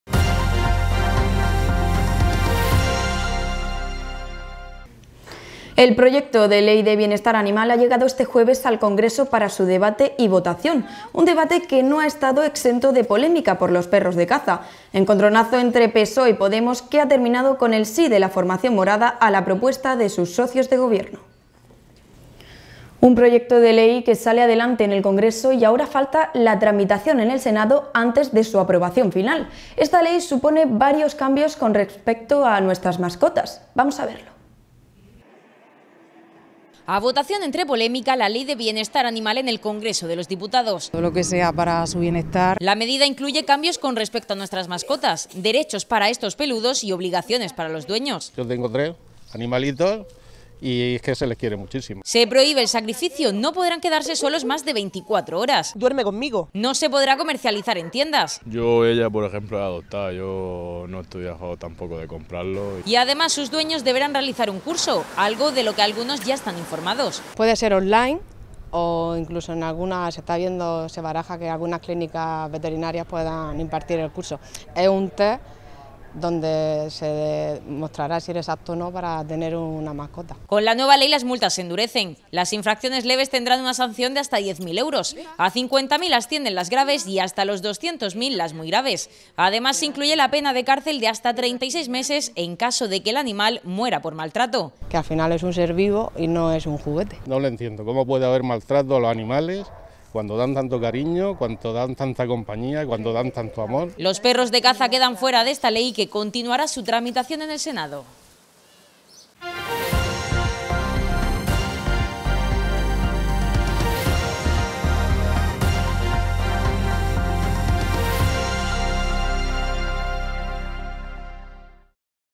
El Congreso aprueba 'in extremis' una ley de bienestar animal que excluye a los perros de caza
El encontronazo de PSOE y Podemos por la ley de bienestar animal ha terminado con el sí de la formación morada a la propuesta de sus socios de gobierno. Les contamos todos los cambios que supone con las mascotas.